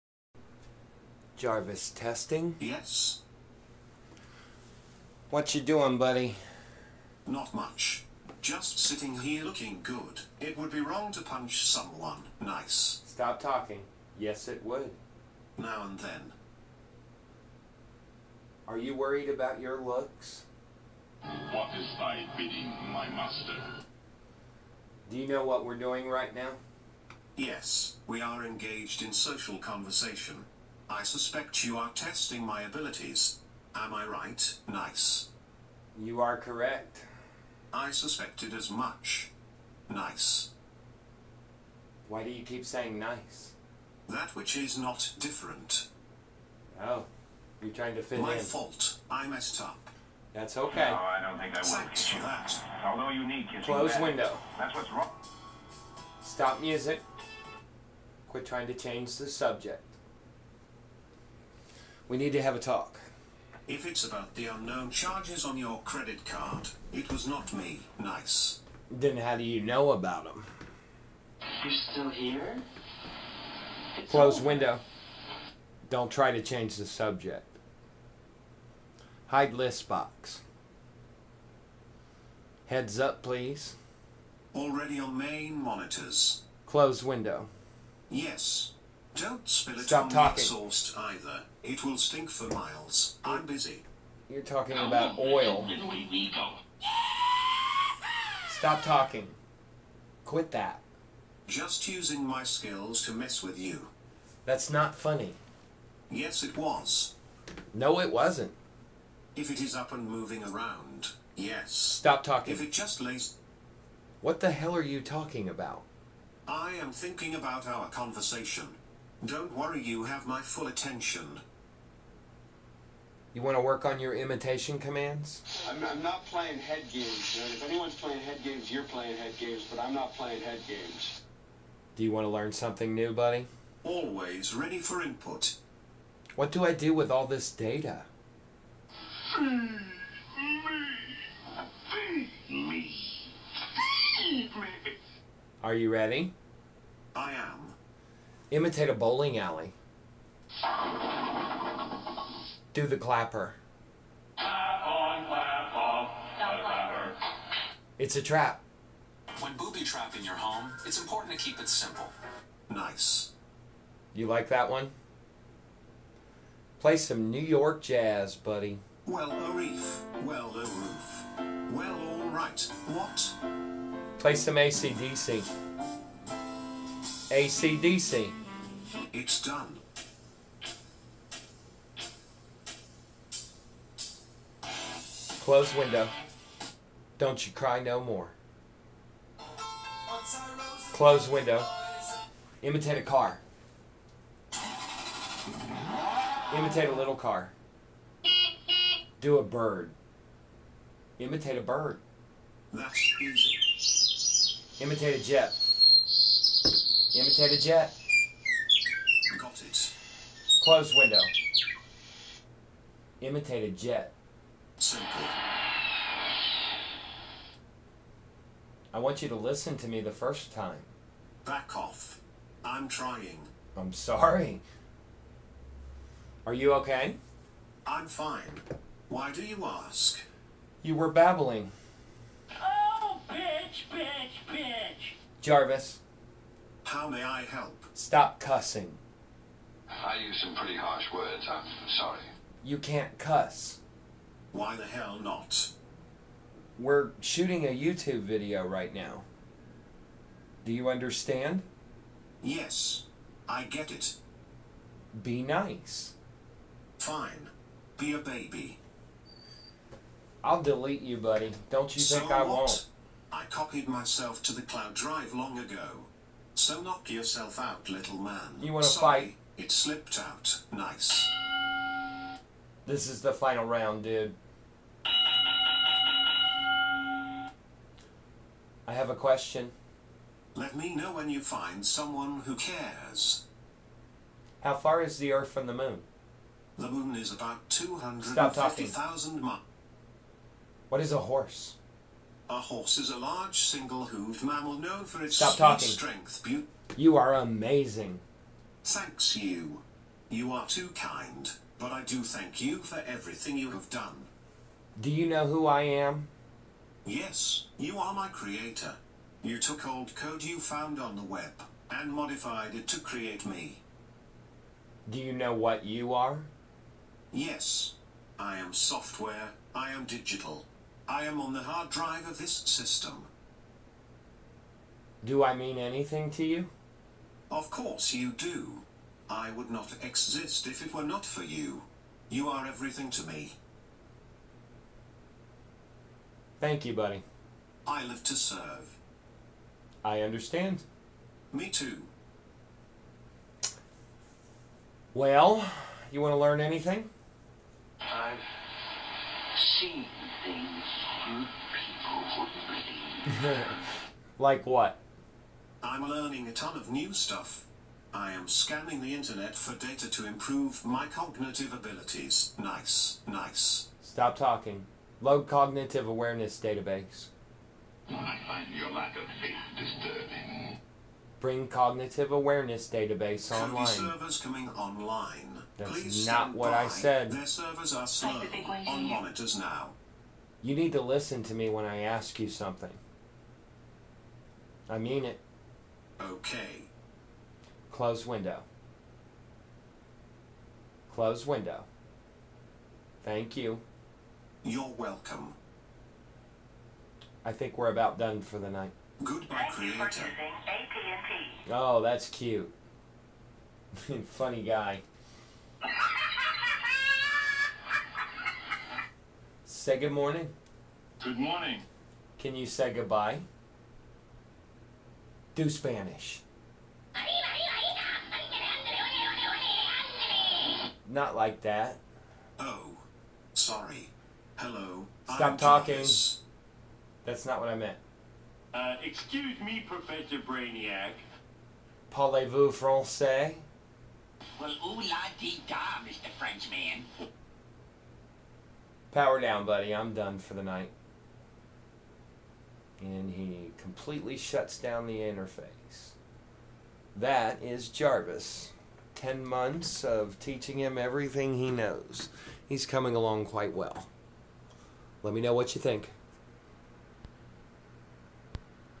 Jarvis Talking Computer
This is my home office. This is my personal system where I've been working on my A.I's code.  He is using only 3 percent of the power of this system.  He is 10 months old now.
My J.A.R.V.I.S Setup with Michael C's JARVIS Program